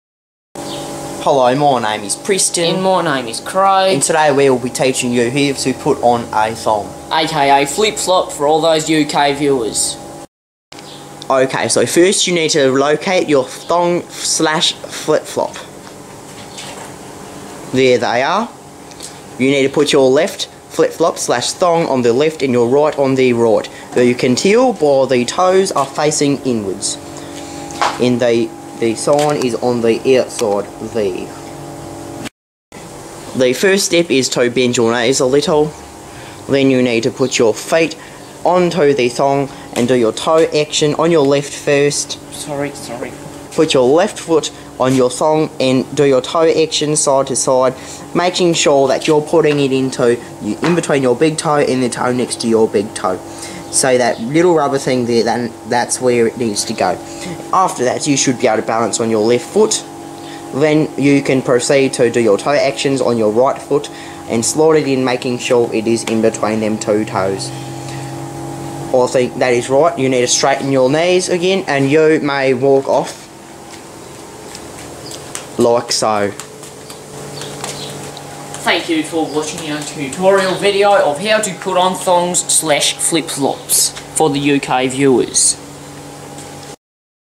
a funny tutorial on how to put your thongs on
a tutorial by Preston and Craig showing you guys the correct and simpler procedure on how to place your foot in a thong/flip flop. we hope you have learnt a lot from this video as it took us a lot of time and dedication to come up with a procedure like this one. please do enjoy and we both do really hope you have a bright future with putting your thongs/flip flops on.